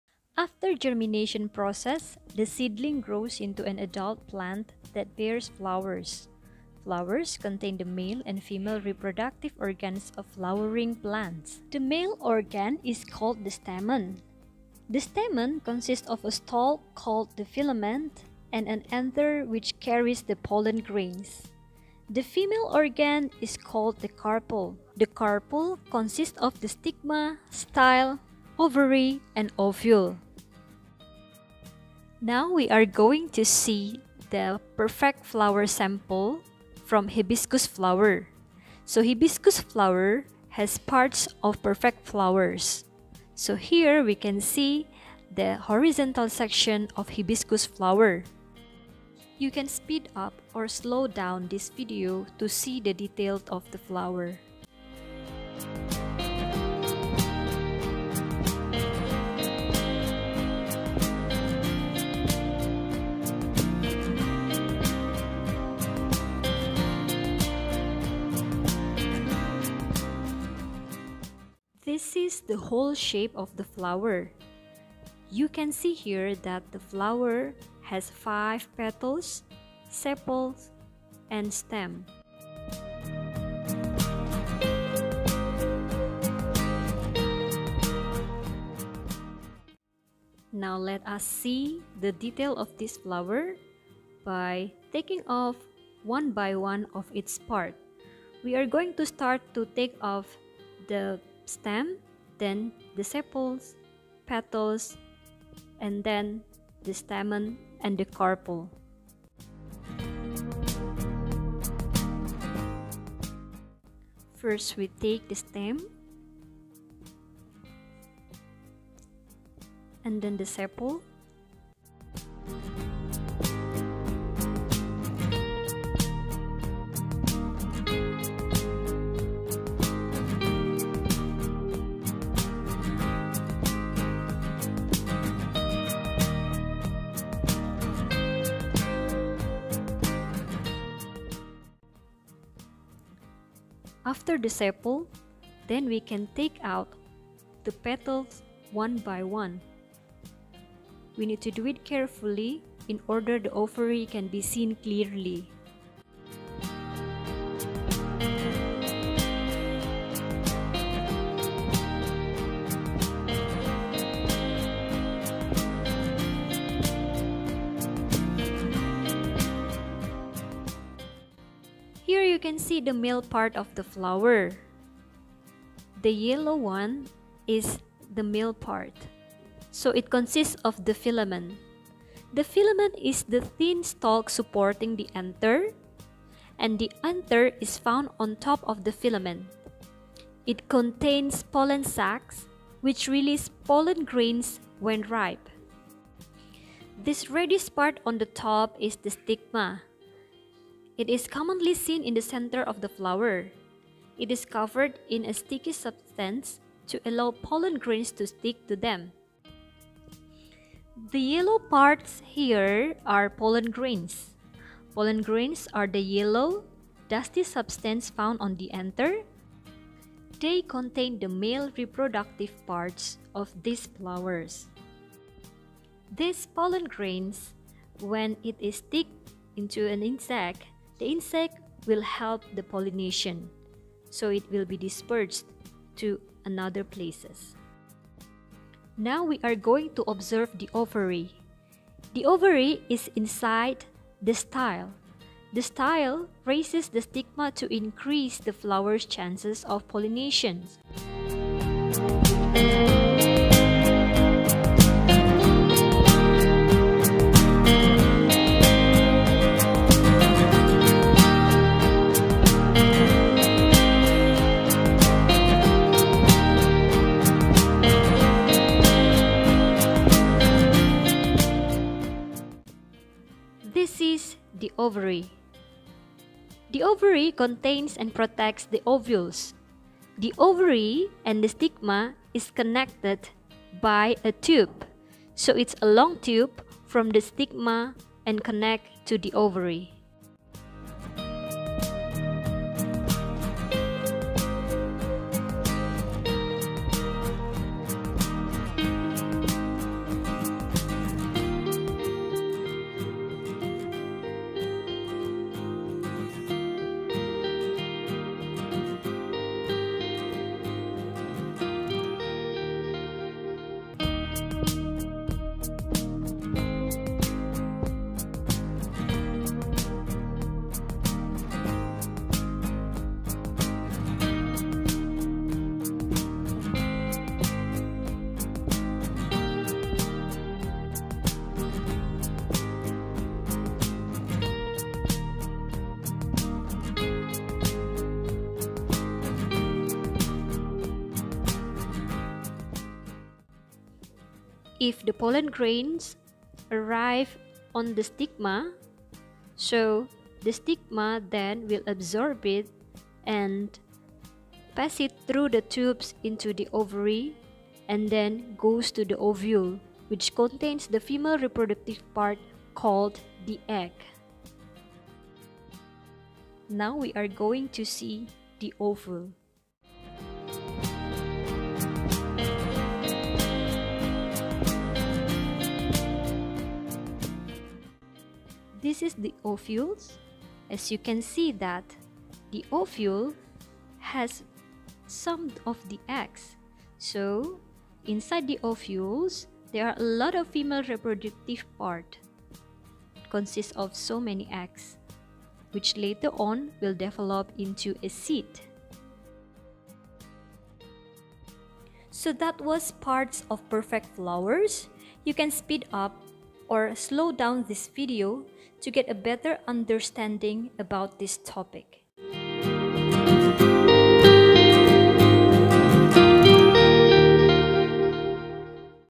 Parts of perfect flowers ; Dissection of Hibiscus Flower | Flower Production
This video discusses about dissection of hibiscus flower explaining about parts of perfect flower.